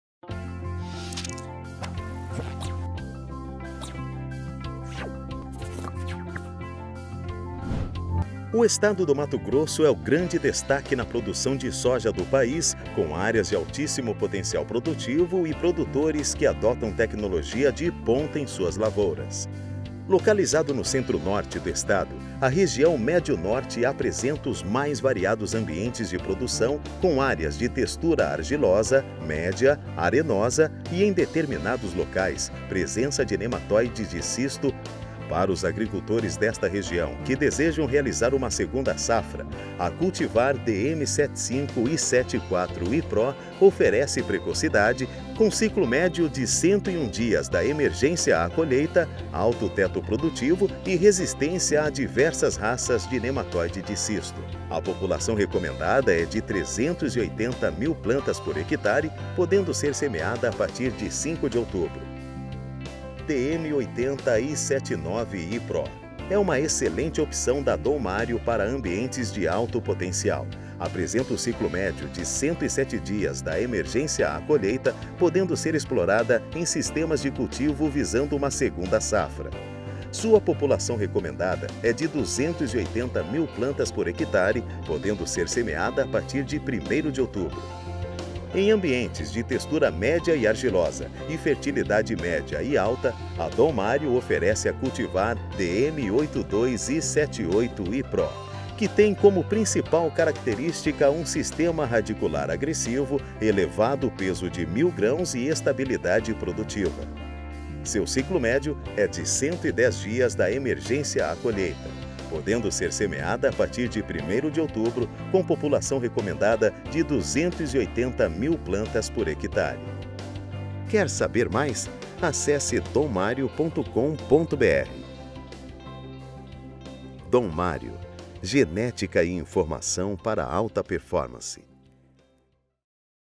Cultivares de soja para o médio norte- MT
Assista ao vídeo e conheça o posicionamento ideal das cultivares de soja DONMARIO com recomendação para o médio norte do Mato Grosso !

DONMARIO, genética e informação para alta performance!